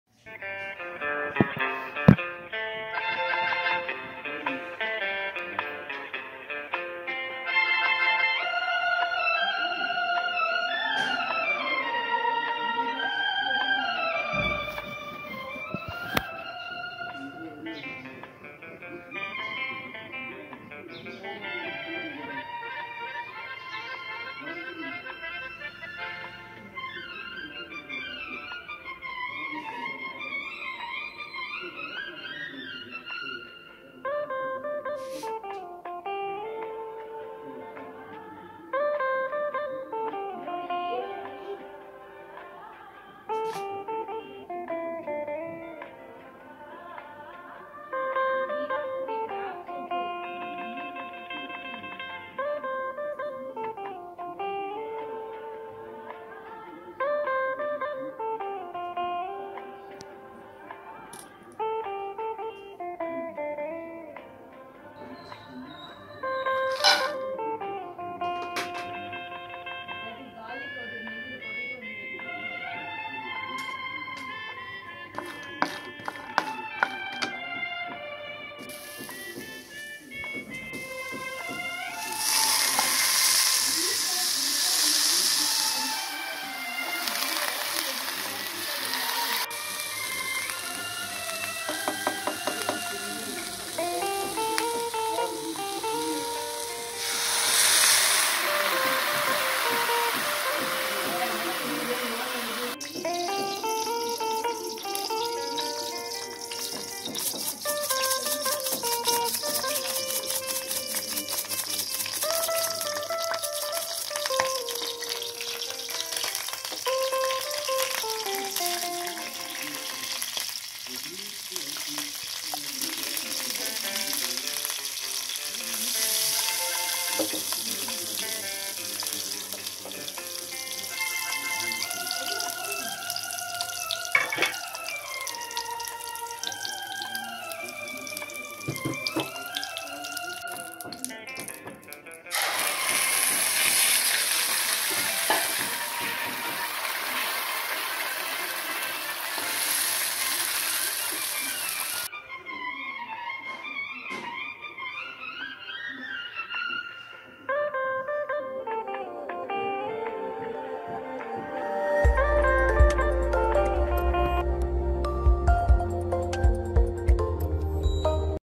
SPICE C TASTE T PEA NUT CHUT NEE
Easy n fast to cook, Healthy n Spicy/ Tasty Peanut chutney.